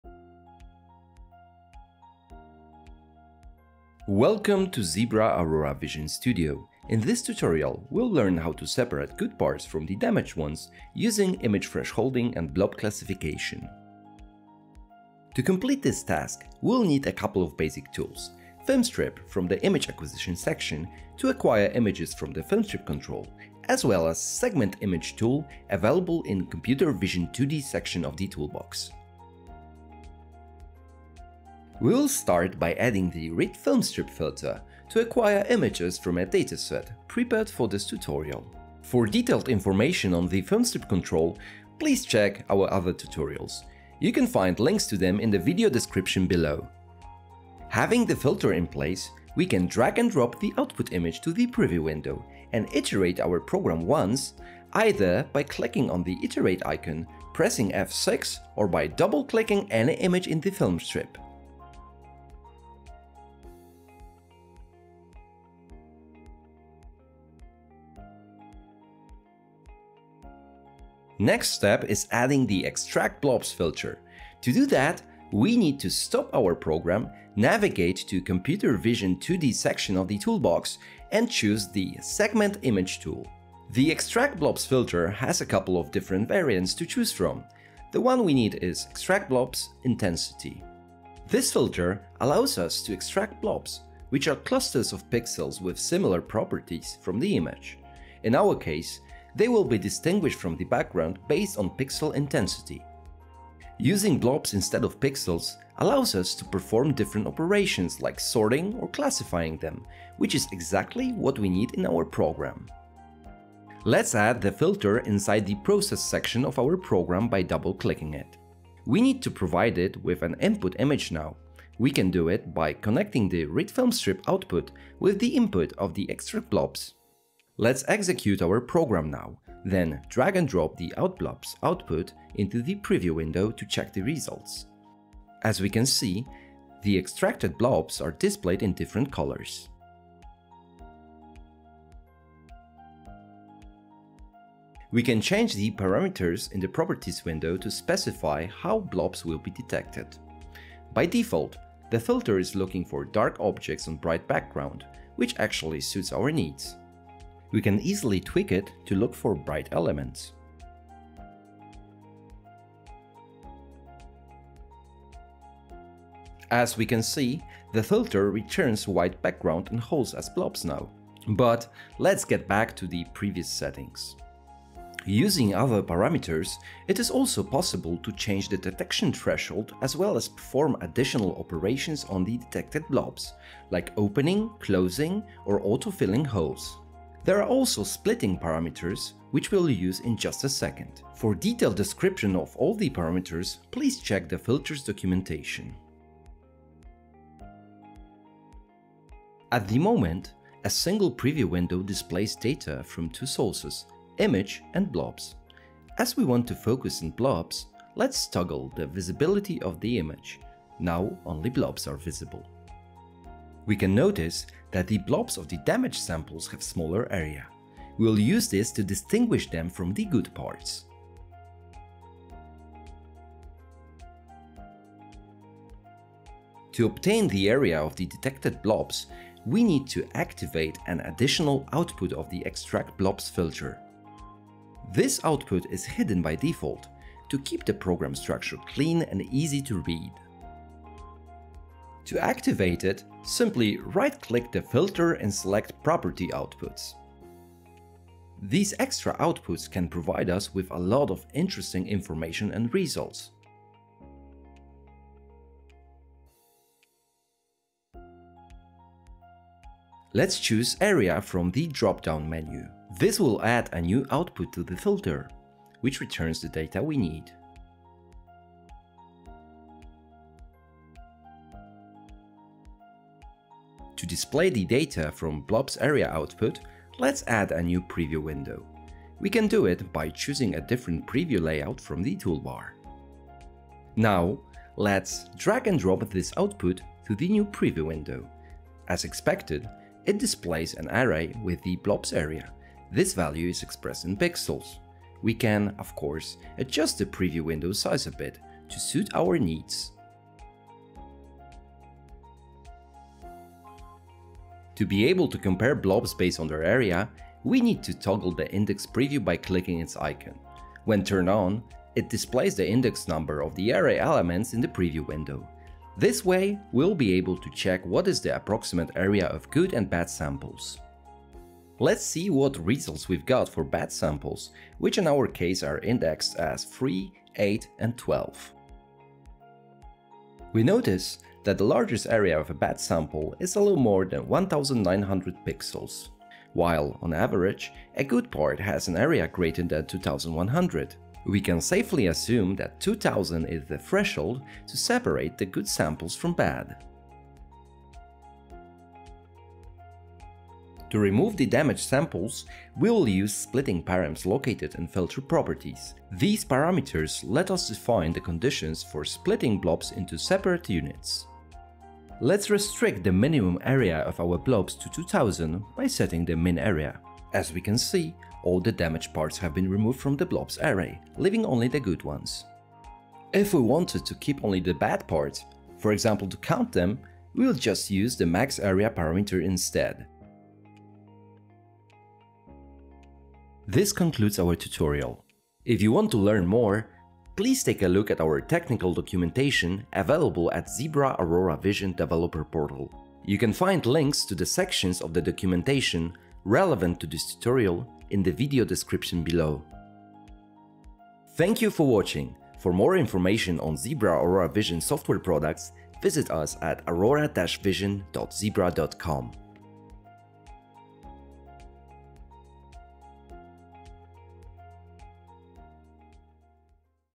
Program Example | Count & Classify Mounts | Zebra
In this example, we will learn how to count & classify mounts using image thresholding and blob classification in Zebra Aurora Vision Studio™.

Contents:
00:00 Introduction
00:14 Used Tools
00:30 Acquiring Images
01:08 Image Thresholding & Extracting Blobs
02:22 Setting ExtractBlobs Parameters
03:17 Adjusting Previews & Activating Property Outputs
04:32 Displaying Blob Area
05:02 Activating Index Preview & Classifying Blobs
05:48 Final Steps
06:22 Documentation & Links

For the technical documentation for all Zebra Aurora Vision™ software products, as well as other resources, visit Zebra Aurora Vision™ Developer Portal